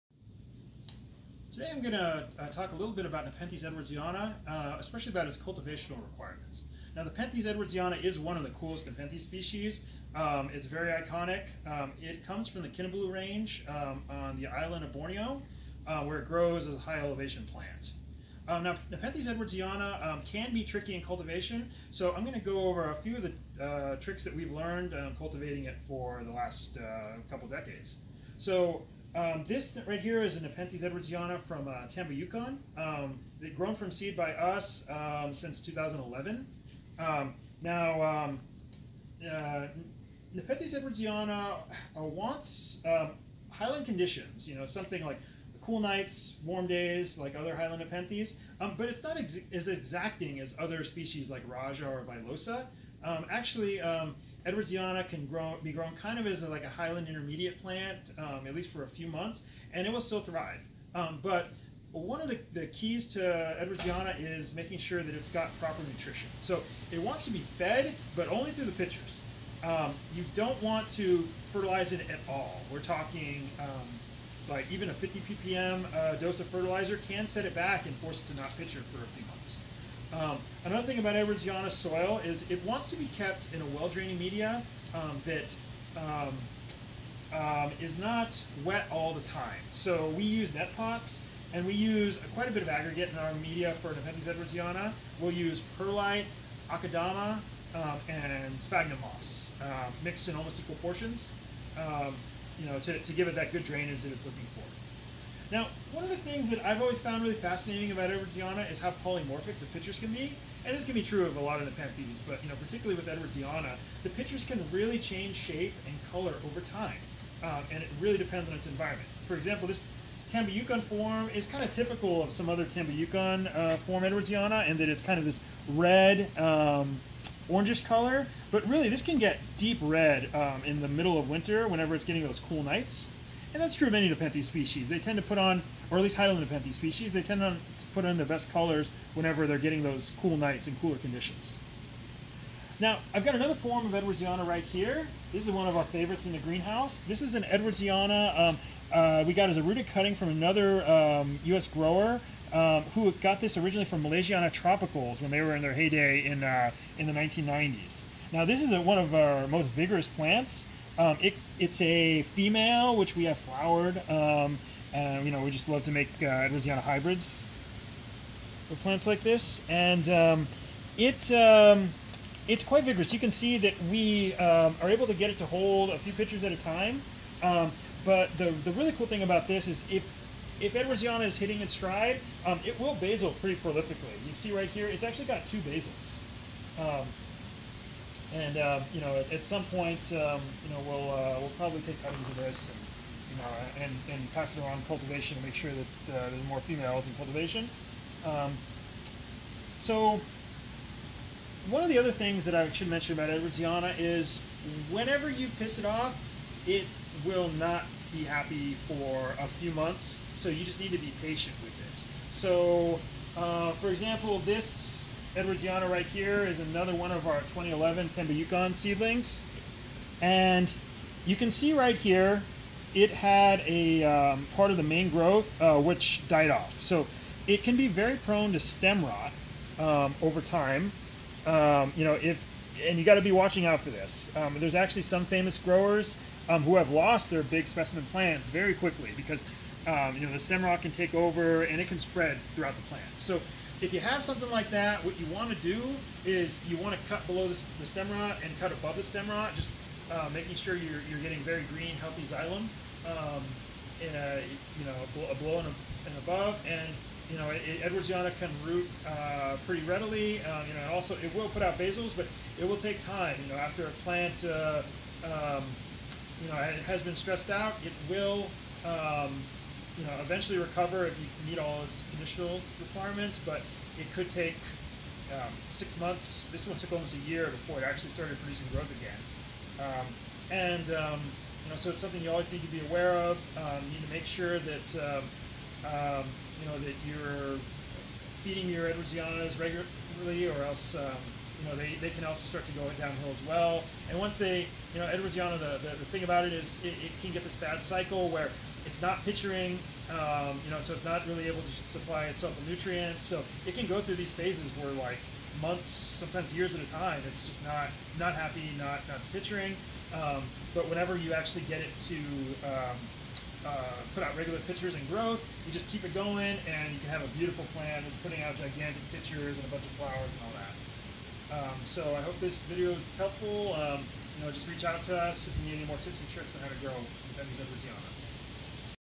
Nepenthes edwardsiana Cultivation
We describe some tips and tricks for cultivating the iconic Nepenthes edwardsiana based on our experience.